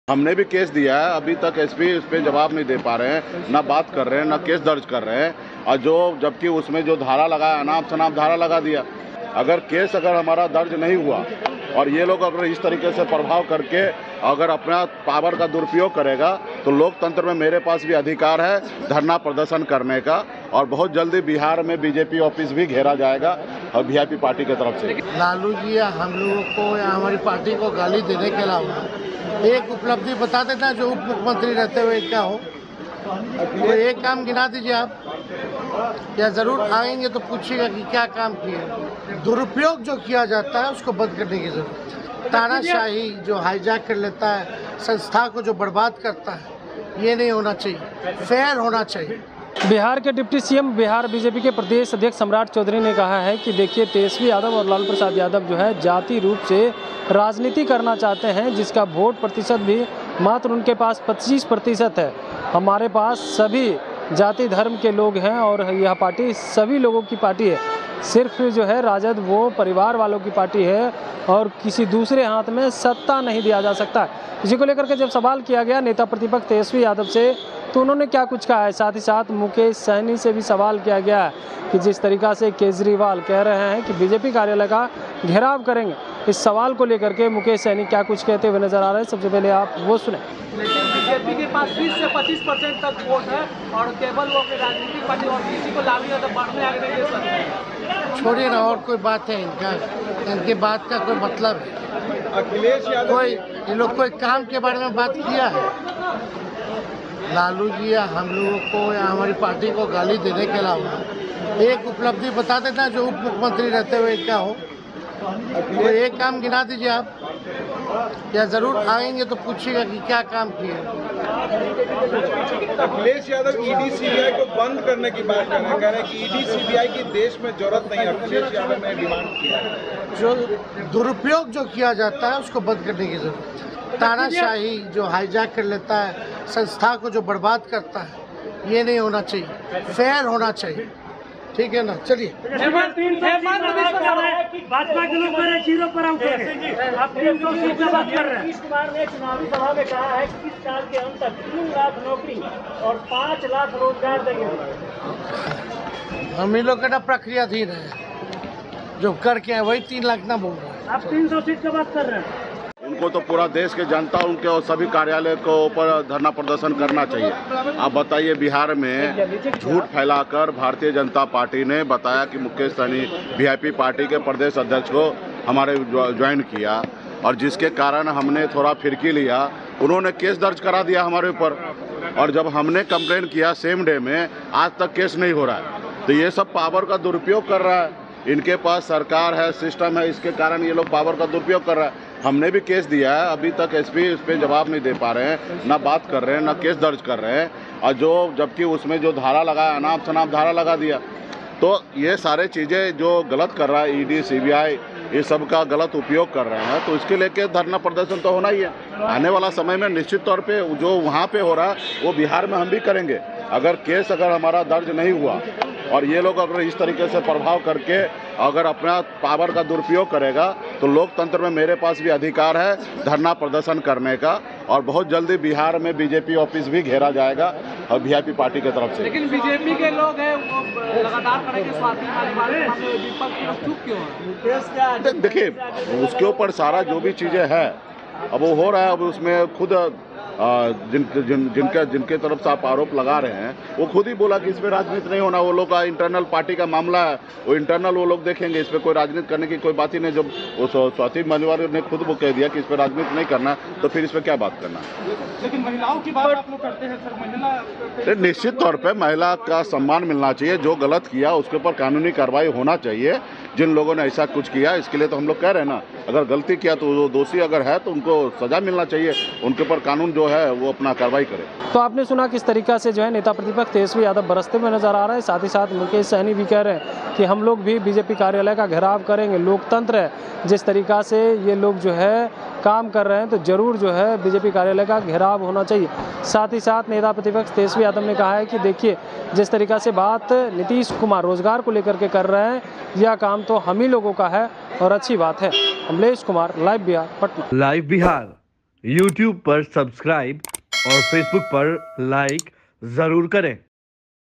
Tejashwi के साथ मुकेश सहनी ने कर दिया ऐलान, बोले-सम्राट चौधरी को छोड़ेंगे..BJP कार्यालय का घेराव...
Live Bihar Digital Network
Tejashwi के साथ मुकेश सहनी ने भी कर दिया ऐलान, बोले-सम्राट चौधरी को छोड़ेंगे नहीं..BJP कार्यालय का घेराव...